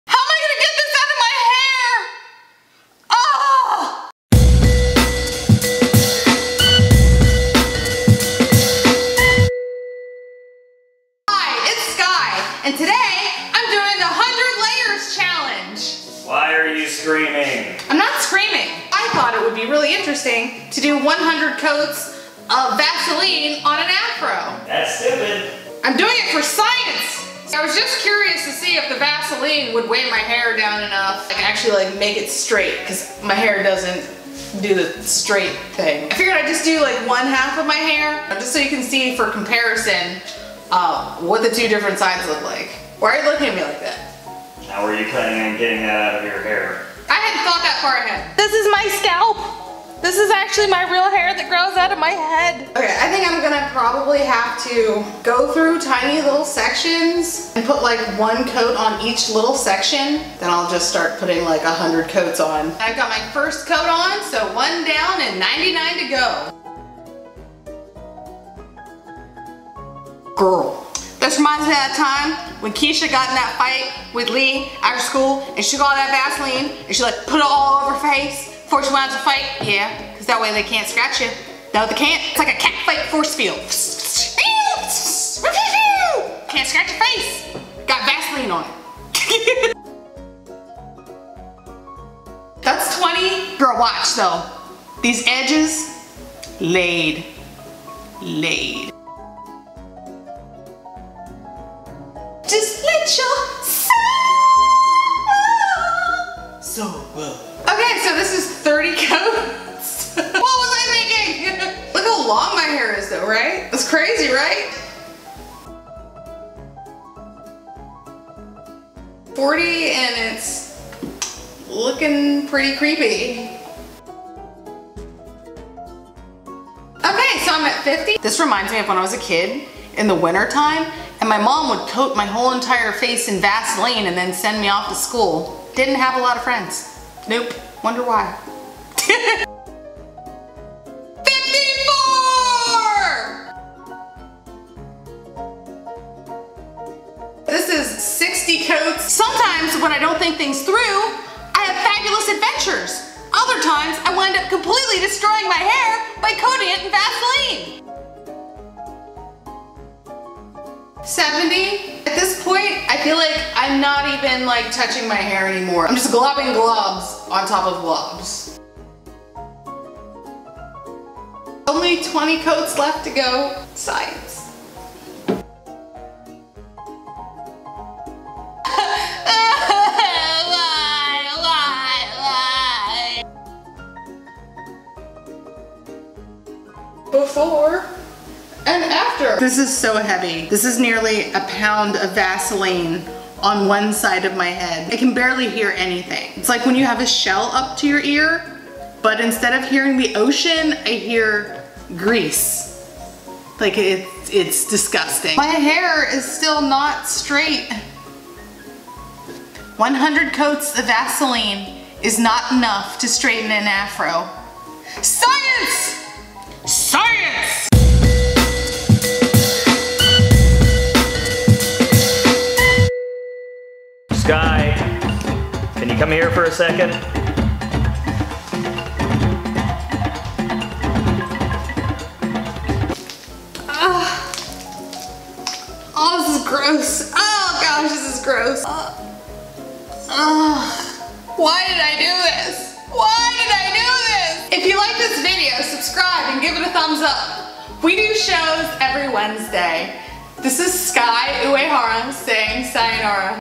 ✔️ 100 Layers of Vaseline on an Afro Challenge (Natural Hair)
100 Layers of Vaseline on an Afro Challenge (Natural Hair) is a 100 layers challenge or 100 coats challenge video.  I put 100 layers of Vaseline on my natural kinky Afro textured hair as a science experiment.  Will 100 layers of Vaseline straighten Afro textured hair?  Watch and see!